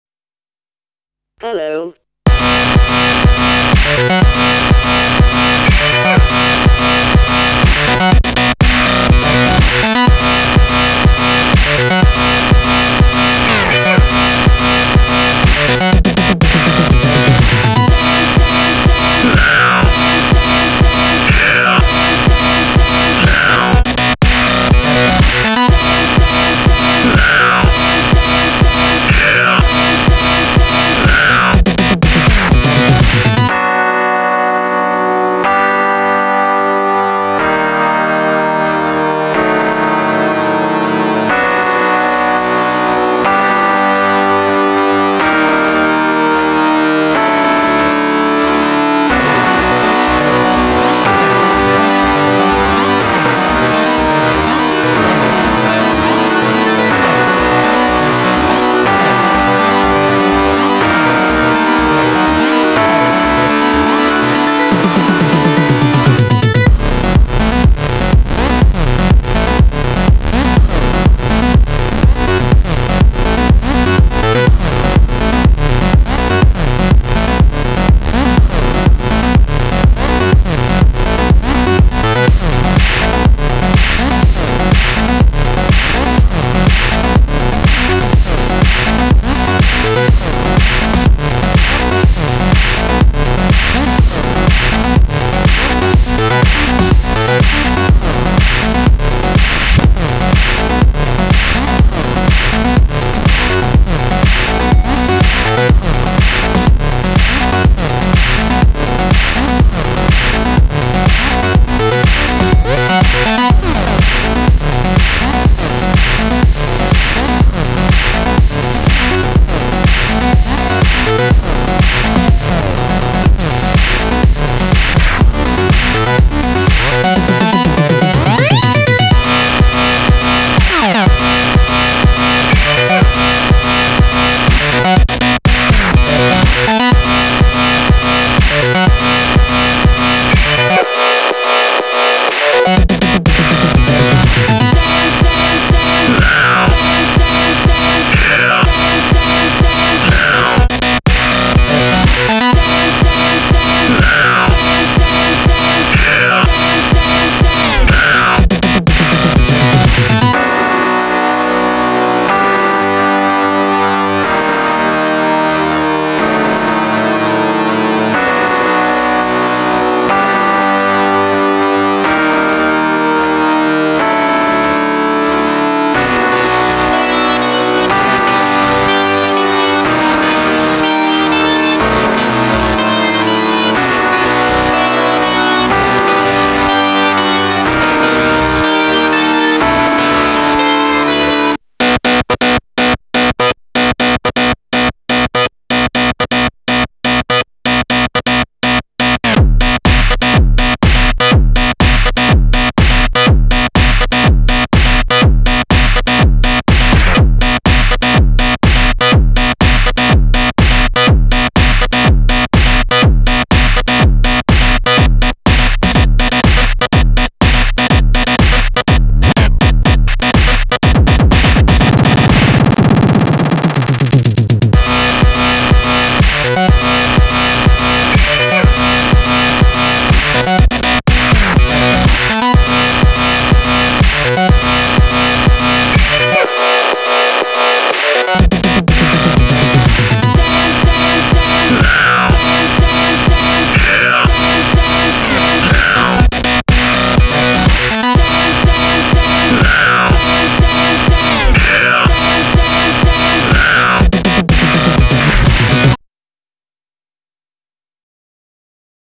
HPI E 10 drift
piste du tourlaville mini bolide ( 50 )
chassis hpi e 10 moteur 23T